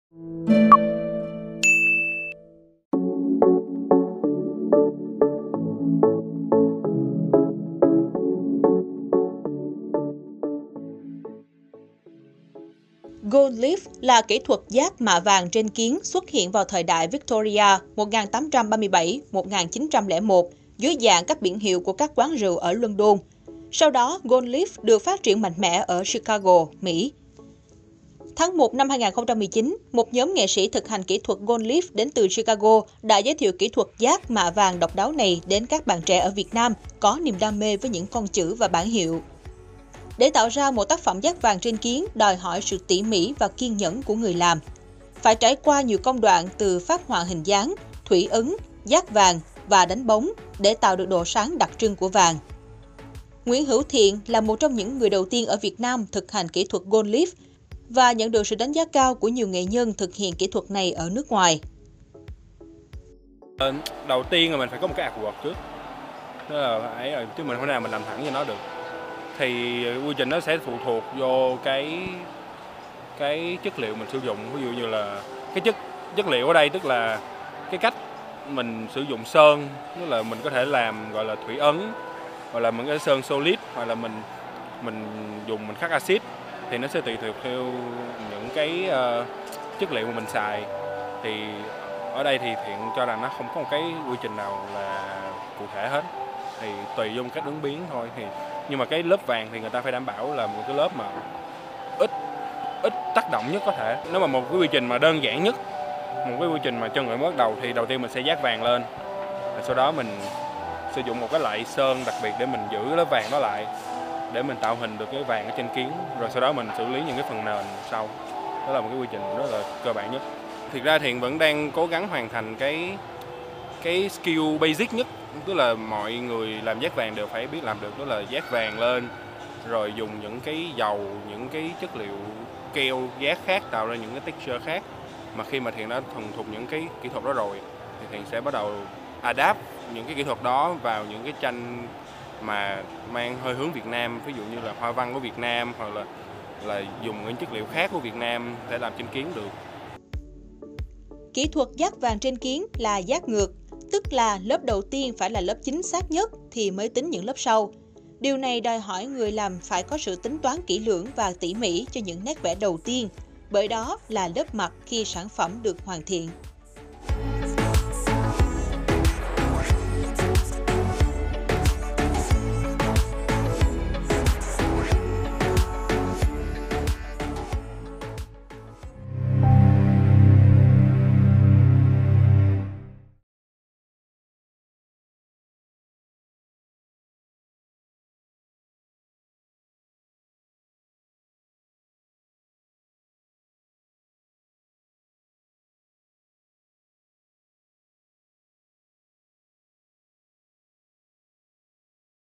NGHỀ LẠ | Dát vàng 9999 lên các biển hiệu ở Sài Gòn
Kỹ thuật gold leaf – dát vàng trên kiếng là một kỹ thuật phương Tây còn mới lạ đối với người Việt Nam, phần lớn gold leaf được thể hiện dưới dạng các biển hiệu.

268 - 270 Nguyễn Đình Chiểu, Phường 6, Q.3, TP HCM